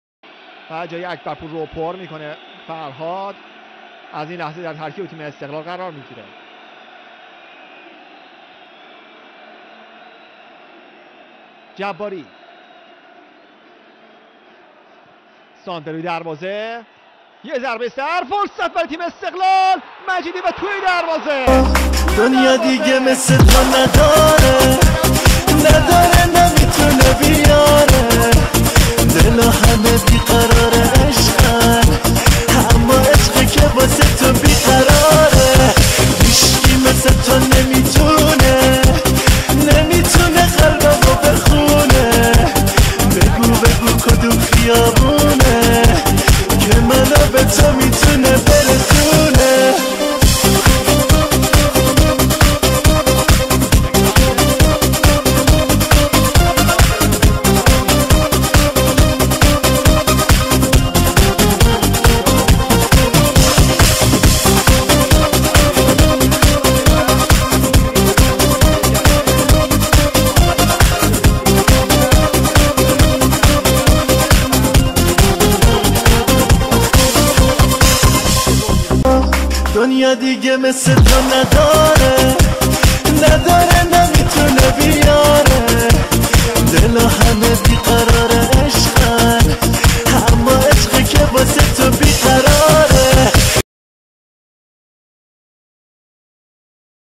Farhad Majidi in 4 Seconds, فرهاد مجیدی
A goal from the legend Farhad Majidi in less than 5 seconds after the restart of the game when he entered the game as a substitute